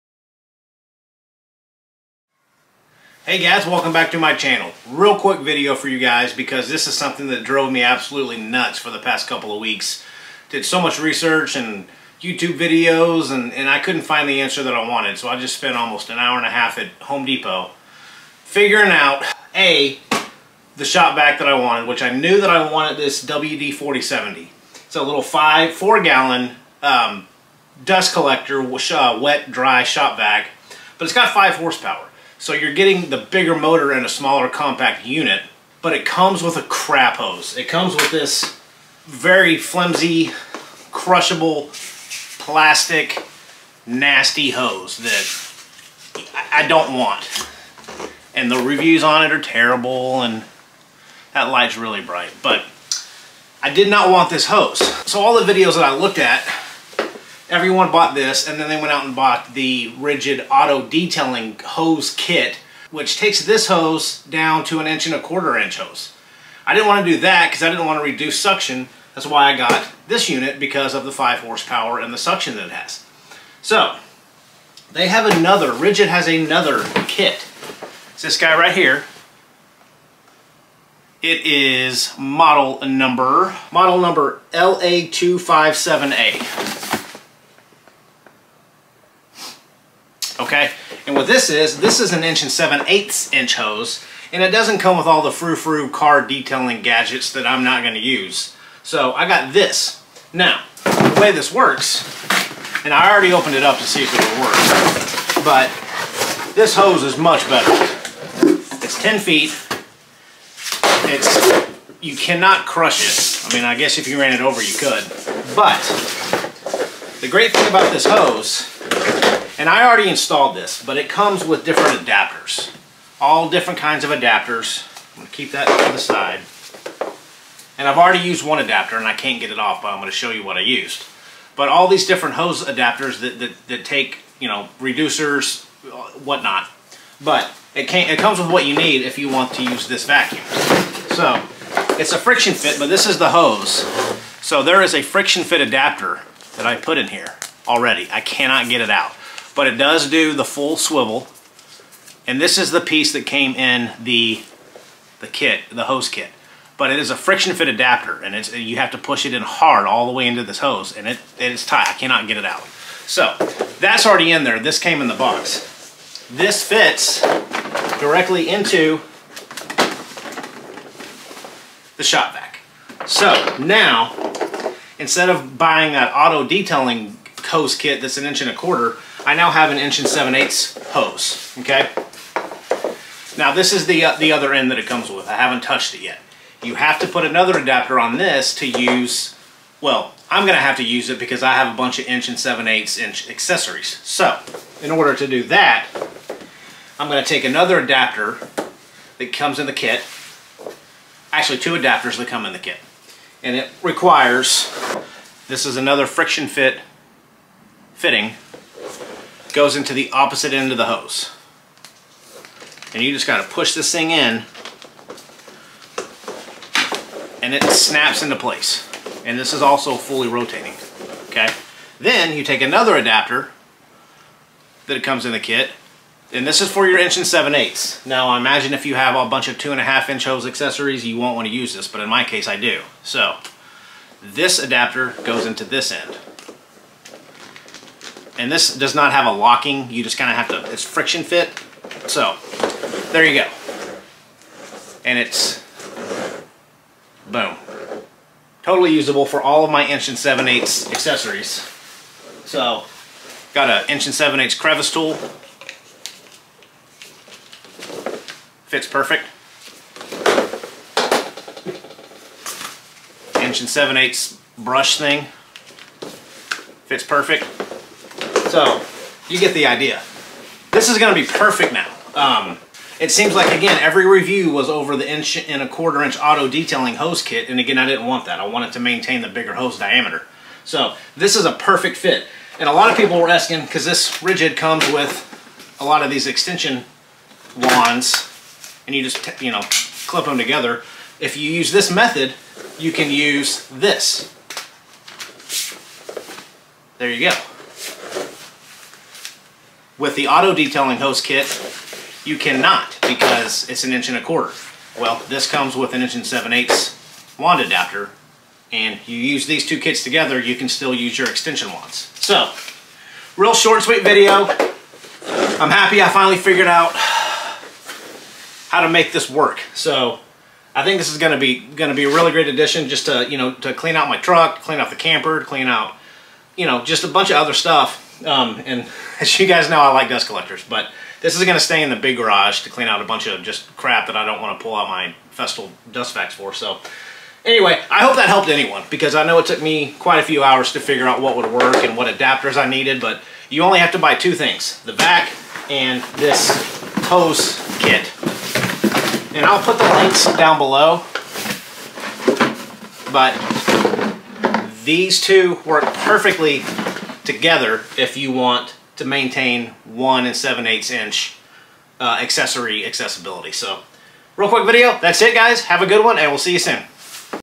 Best hose for Ridgid WD4070! Keep your larger attachments!
Wanted to explain the option I found that I think is the best for this popular Ridgid model. Using this hose kit let’s you use the larger 1 7/8” attachments instead of the smaller 1 1/4’ attachments you have to use if you get the auto detail era hose kit. This also helps maintain the best suction.

Ridgid shop vac WD4070
HOSE KIT MODEL: LA2570